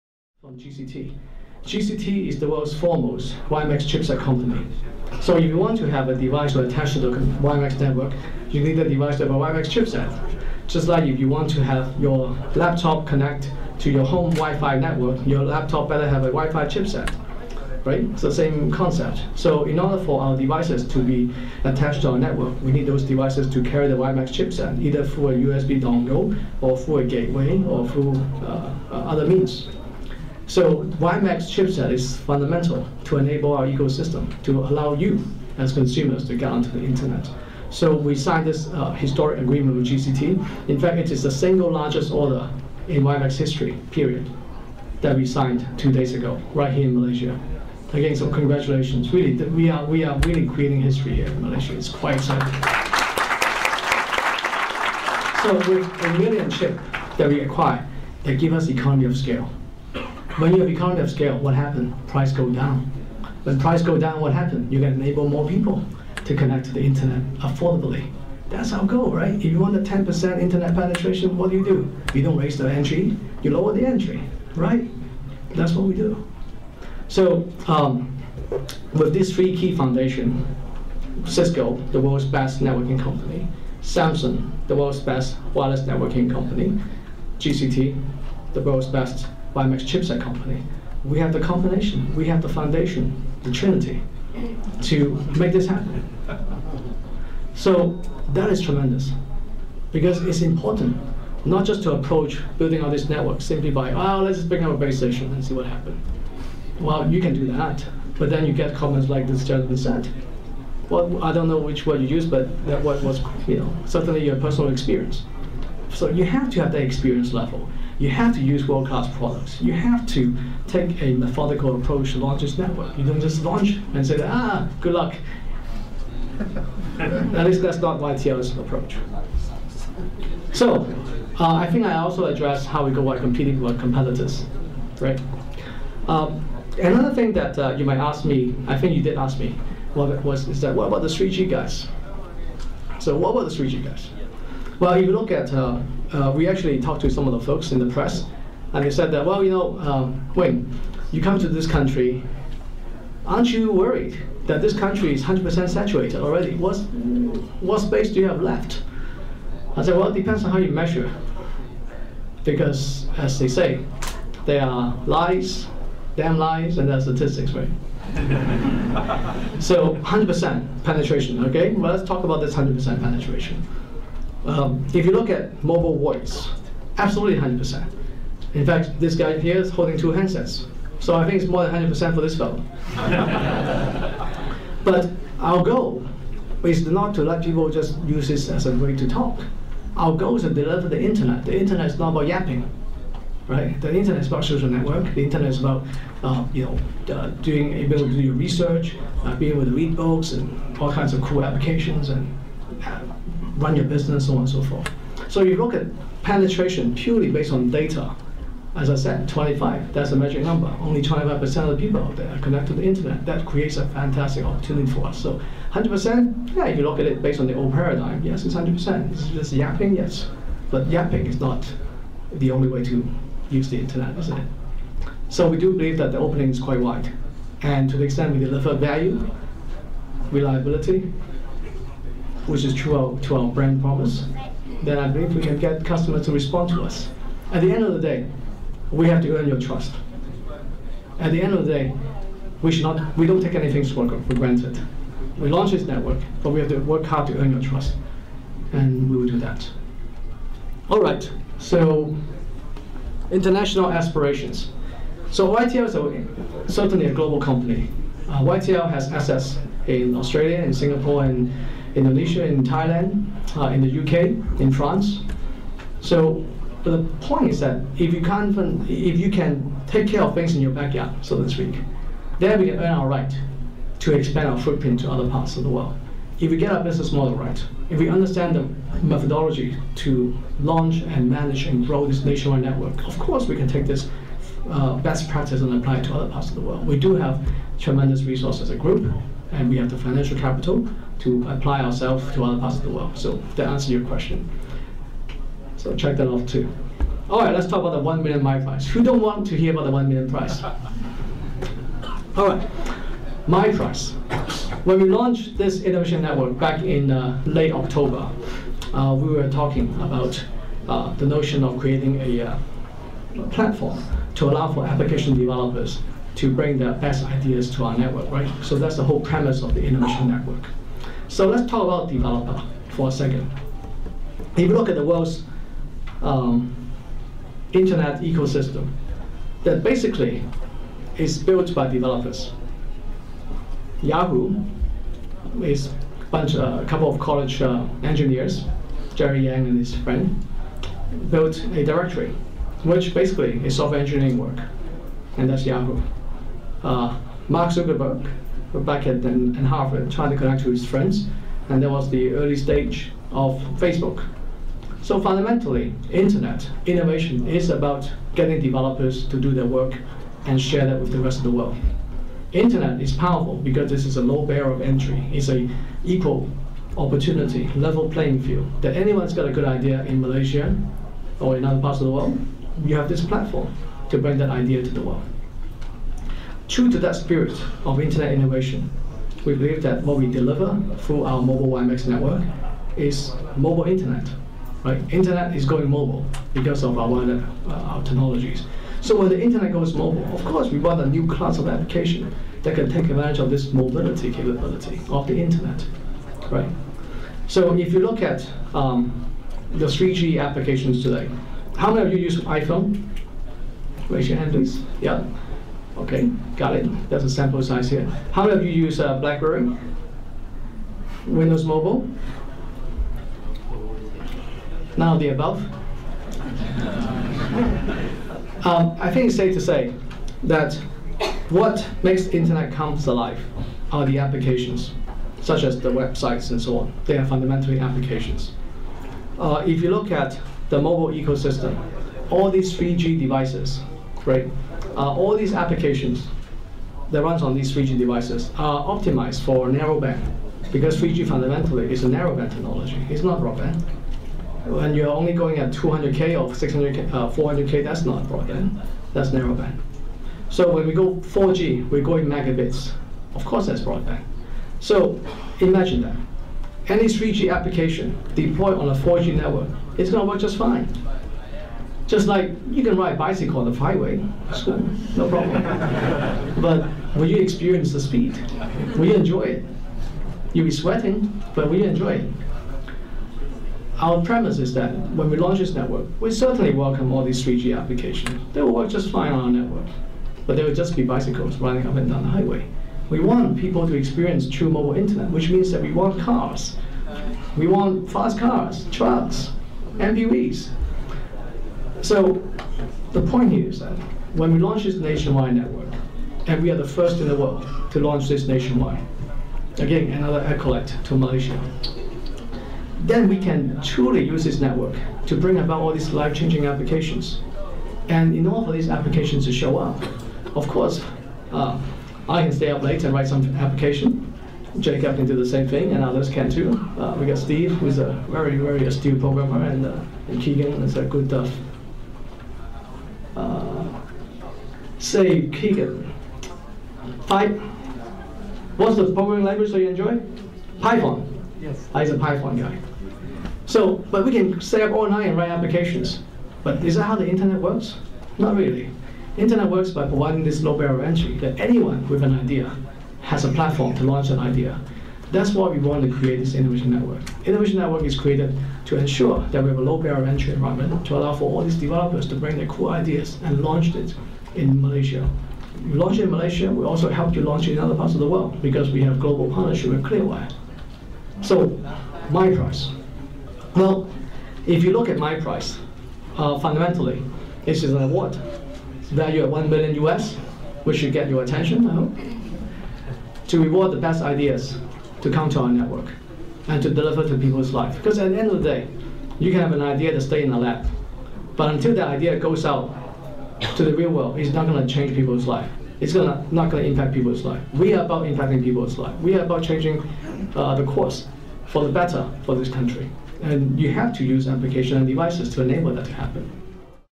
YCM CEO Series 21 - Wing K. Lee (Part 09)
The CEO of YTL Communications speaks on what he's working on for the past year or so, specifically on his 4G network and of course, the much-talked about USD1 million mYprize, a global competition aimed at challenging developers to create original applications and devices for its worlds first nationwide 4G Network. He will also be speaking a little on how he came to be where he is today, which we can probably learn from.

Video shot and edited by Nigel Sia. to find out more.